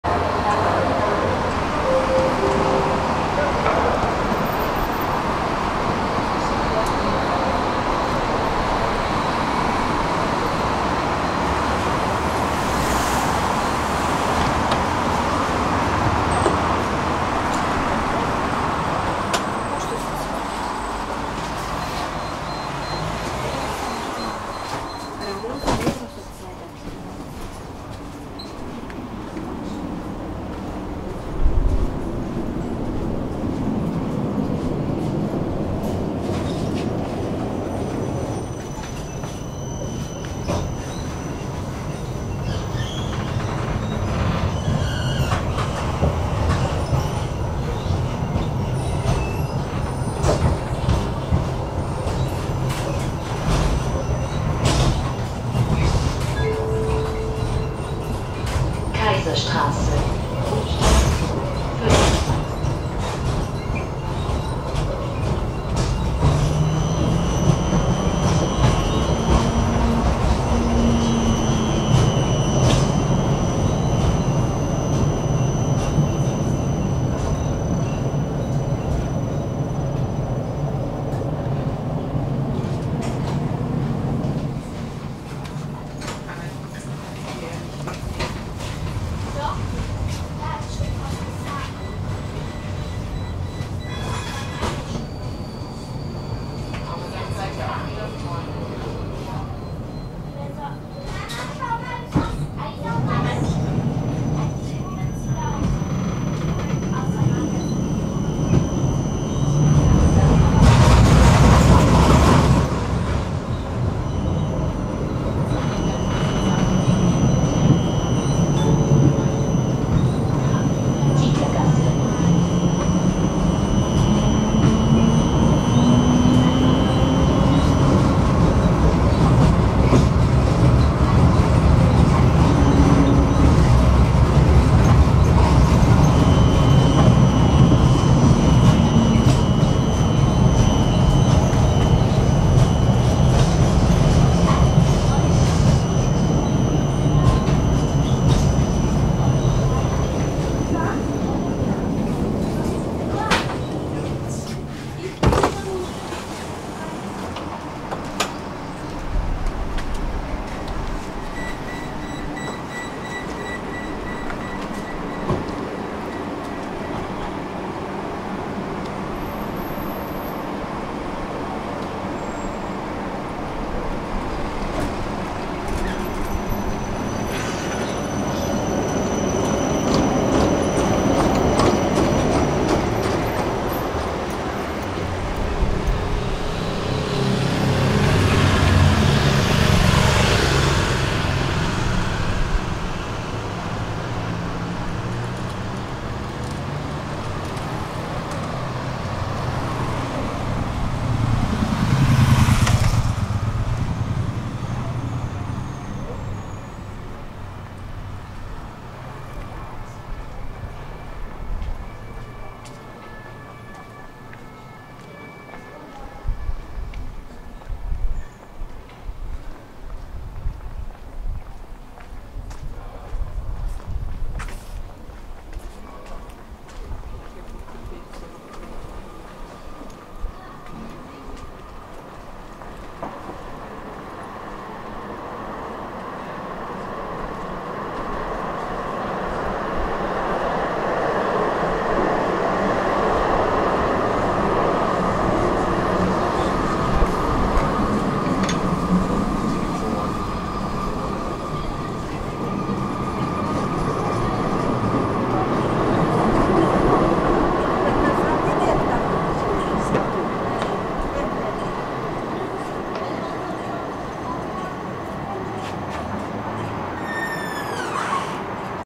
Austria, Vienna, Straßenbahn 49 ride from Urban-Loritz-Platz to Zieglergasse
Straßenbahn 0:02

- recorded with
SIRUI 35mm T2.9 1.6X FF Anamorphic Lens, DJI RS3, Sony ECM-XYST1M

June 2023